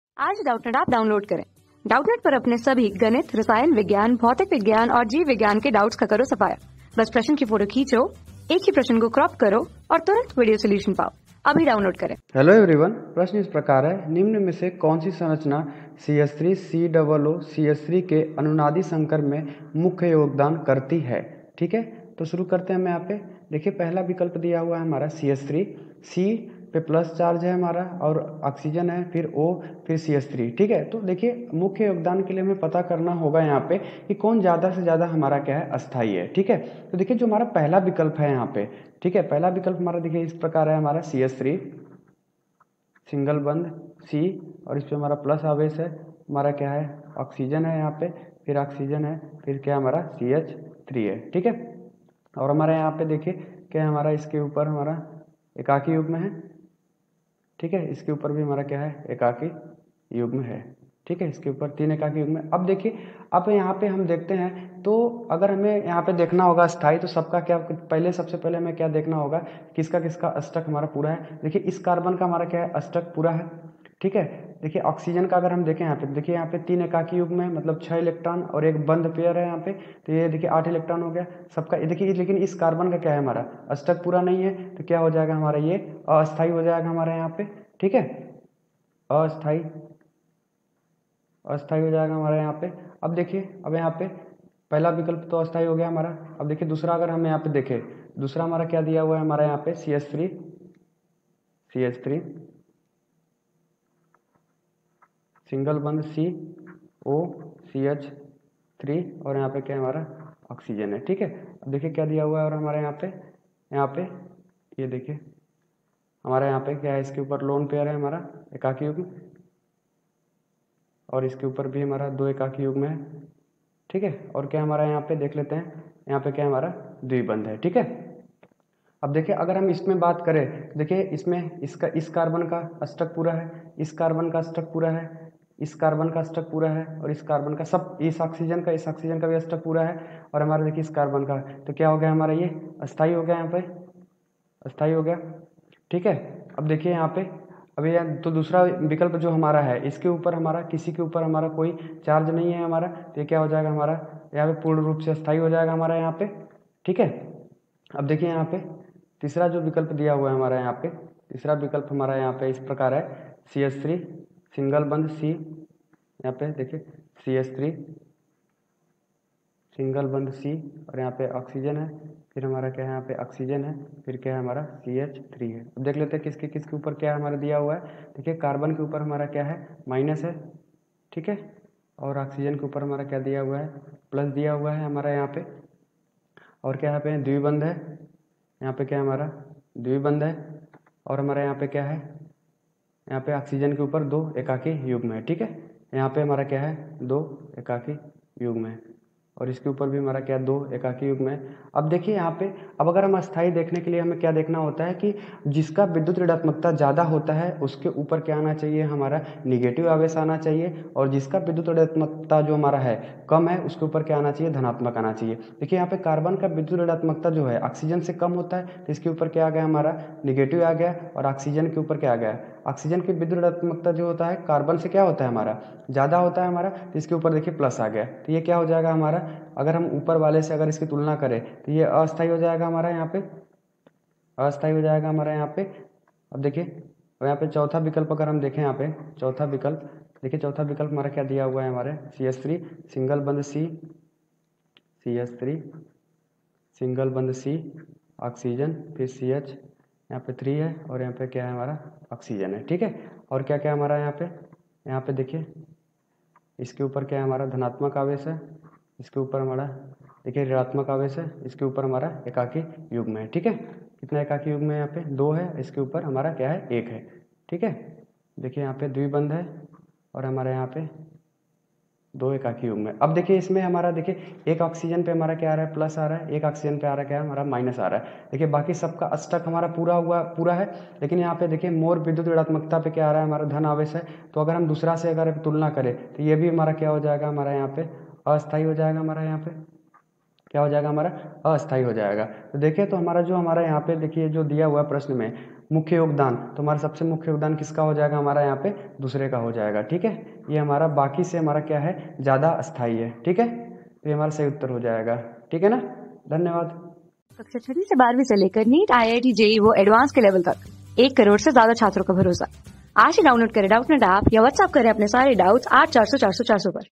निम्न में से कौनसी संरचना CH_(3)COOCH_(3) के अनुनादी संकर में मुख्य योगदान करती है ?ɵ | 12 | DPP...
निम्न में से कौनसी संरचना CH_(3)COOCH_(3) के अनुनादी संकर में मुख्य योगदान करती है ?ɵ
Class: 12
Subject: CHEMISTRY
Chapter: DPP NO. 09
Board:IIT JEE

👉 Have Any Query? Ask Us.